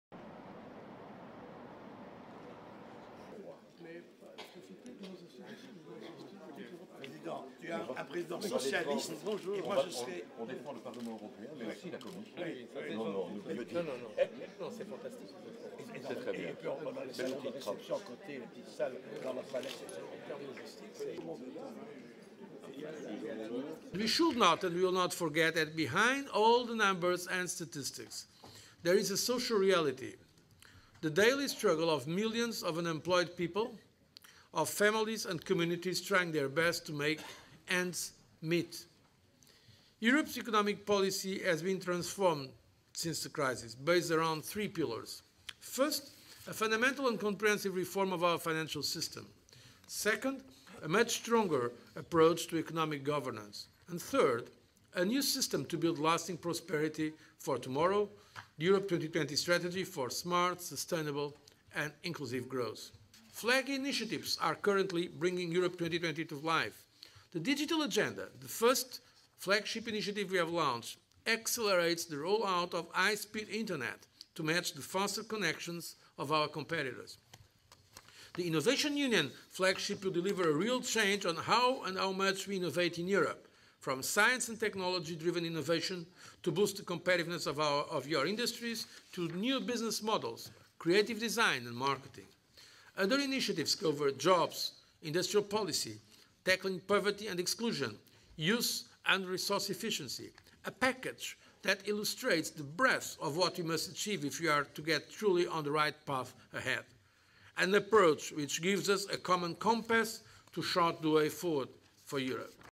Meeting with COSAC
President Barroso attended the Conference of the EU committees of national parliaments and called for strengthened political relationship between the European Institutions and national parliaments, for example on the annual work programme of the Commission and economic governance.  "What really matters is what the institutions mean for the people; whether they can identify themselves with them and stand behind them." He also invited national parliaments to play an active role in the promotion and implementation of the Europe 2020 strategy.